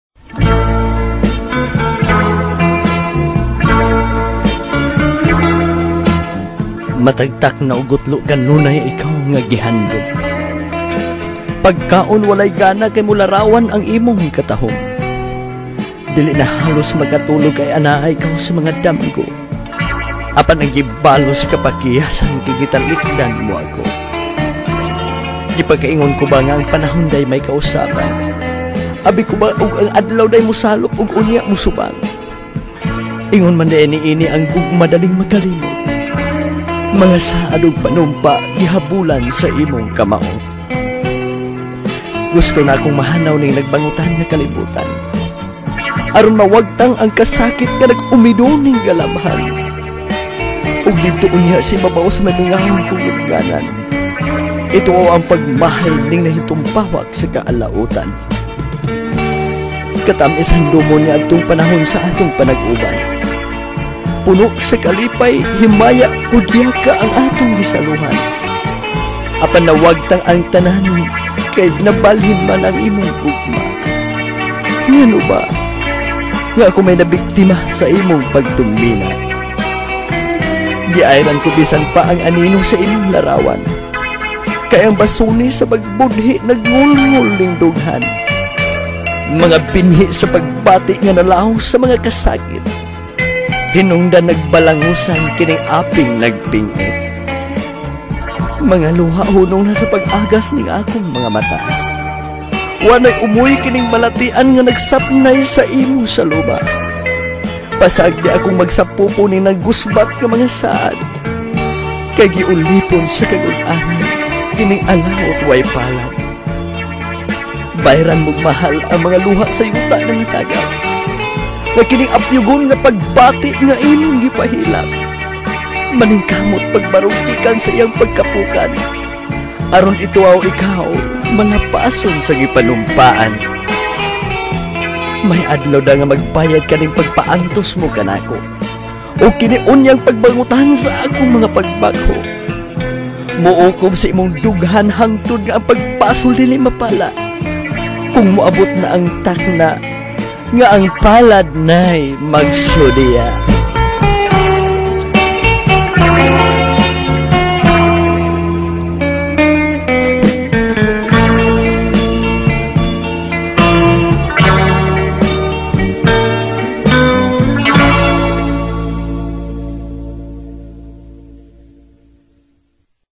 Balak Sa Gugma - Cebuano with Instrumental
Ang Balak Sa Gugma. Gihan-ay ni Julie Bradix Requina Usa ka magbabalak sa DXDD AM Radyo Kampana 657kHz nga nagsibya gikan sa dakbayan sa Ozamiz .